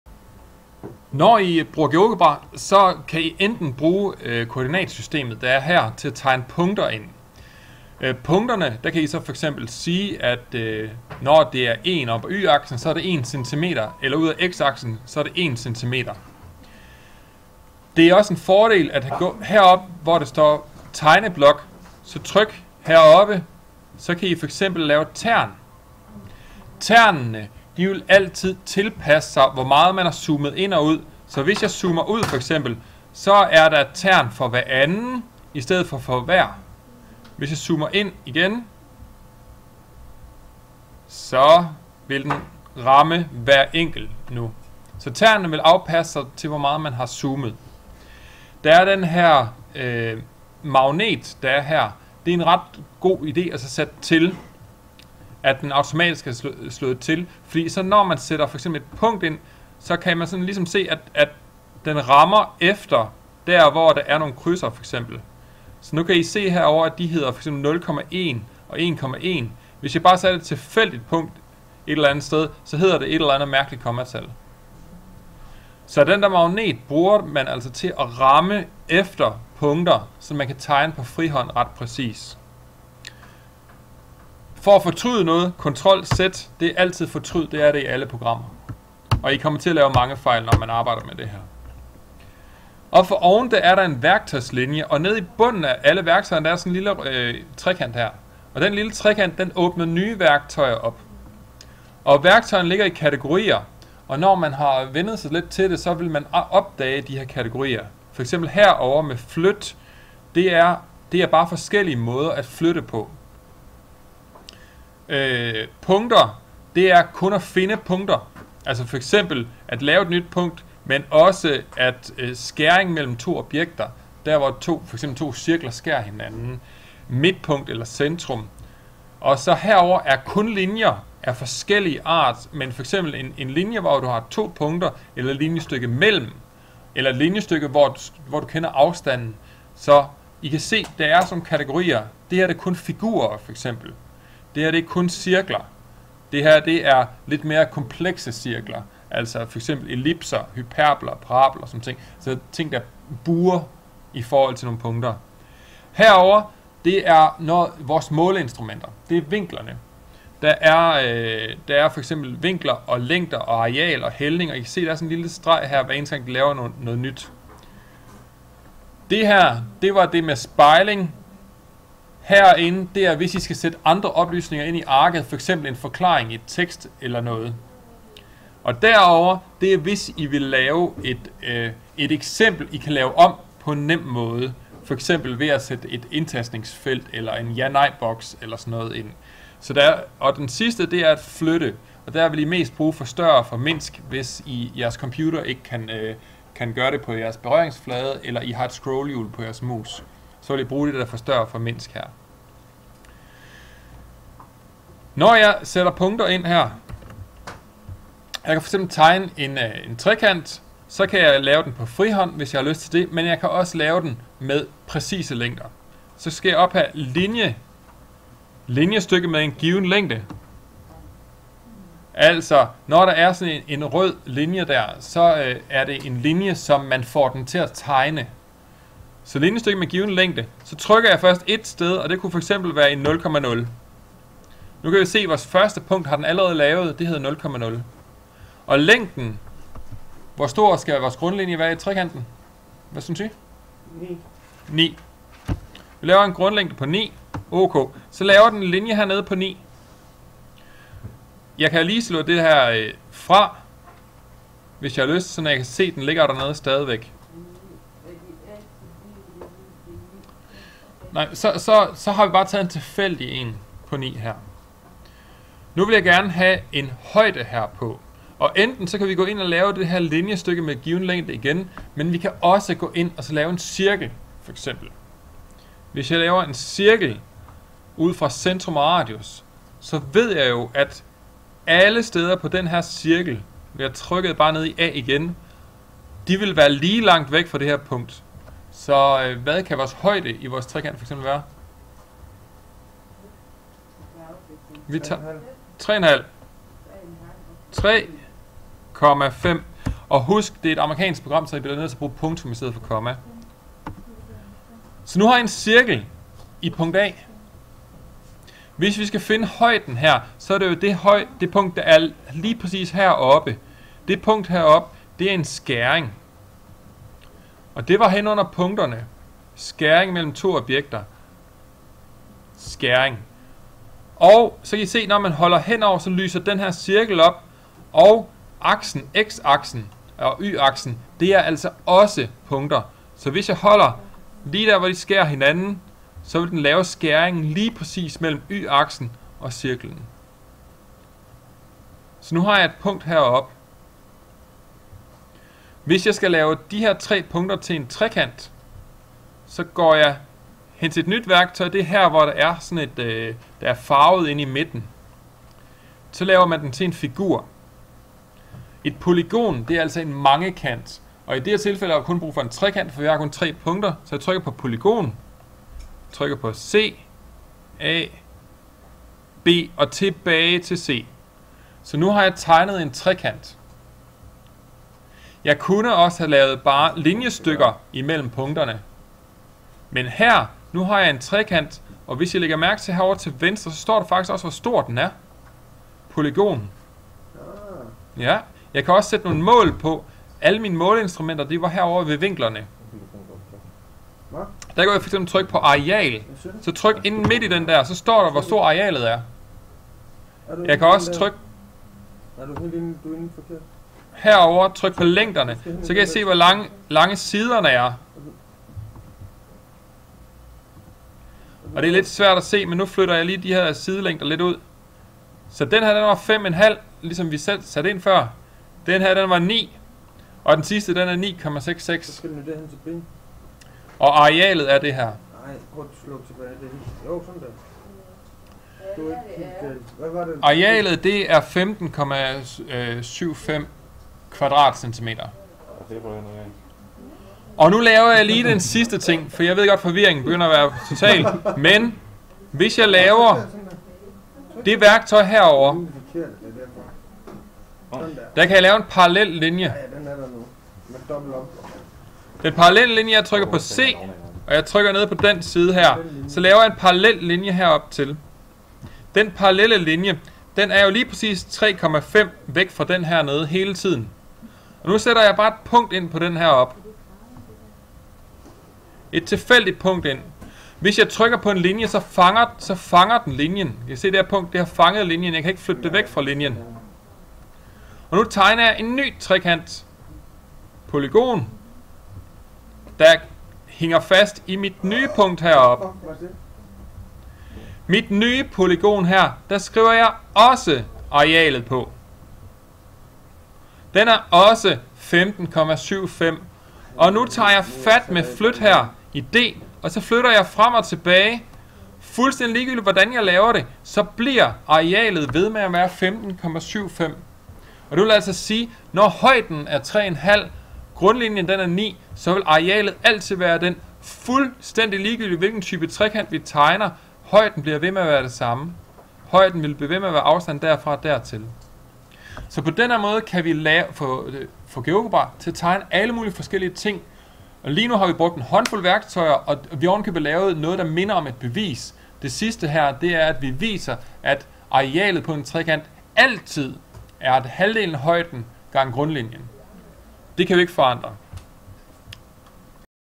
Intro til Geogebra (trekant)
Beskrivelse af en række værktøjer i geogebra med eksemplet at finde arealet på en trekant.
Optagelsen er fra en undervisningsgennemgang med elever.